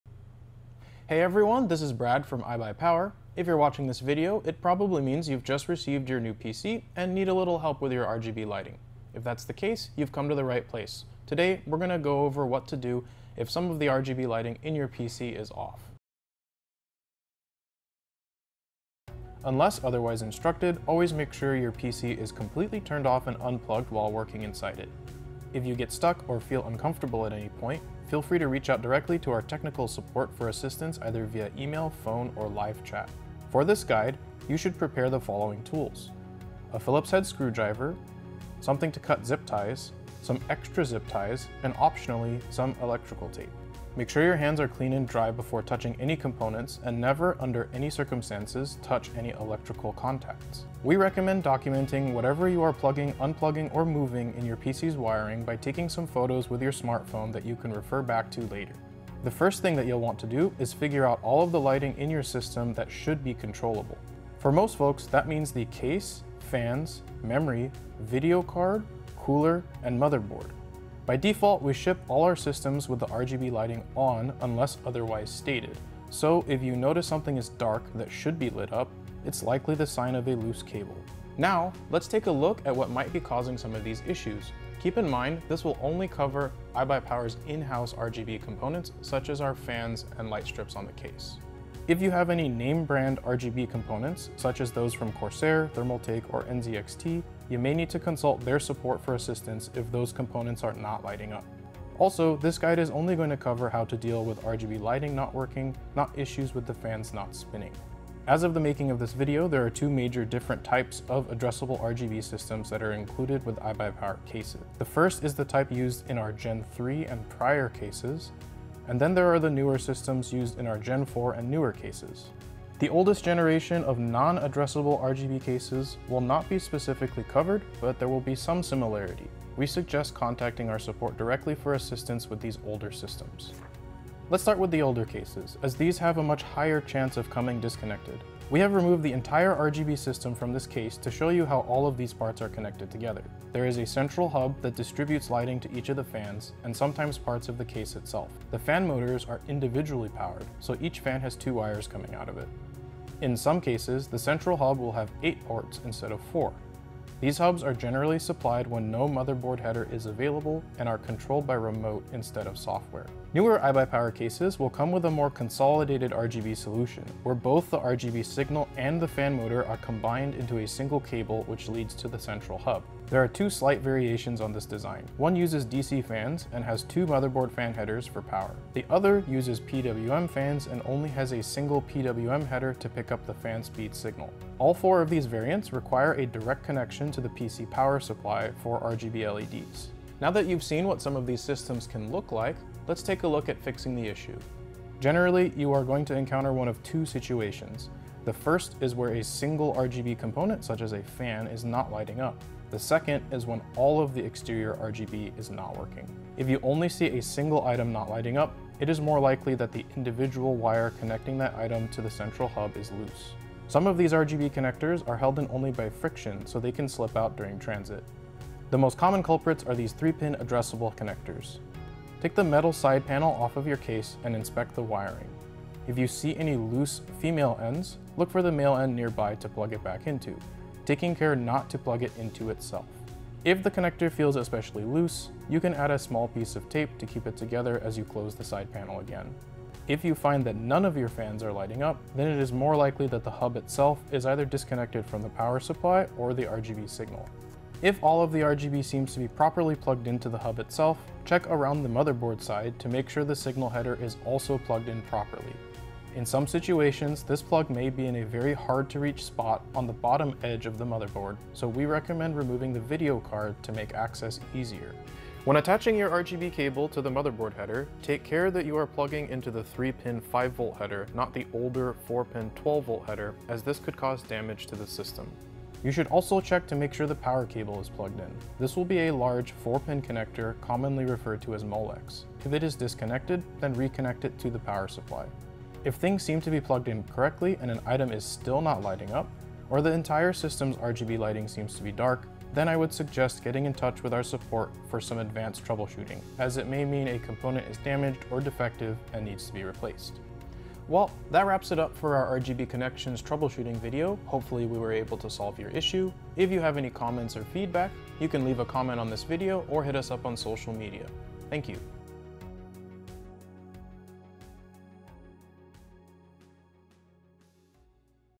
Gaming PC RGB Lighting - Troubleshooting iBUYPOWER Gaming PCs
Your gaming PC RGB lighting is shining bright, but one day you realized one of the lights is off!  Let's troubleshoot reasons your gaming PC RGB lighting could be off.

Video Content

00:00 Intro
00:18 Prep Work
00:58 First Step - Check What's RGB in System
01:31 Name Brand Parts?
01:48 iBUYPOWER System RGB Generations
02:18 aRGB Gen 3 and Prior
02:43 aRGB Gen 4 and Current
03:09 Fixing the Issue
05:00 Conclusion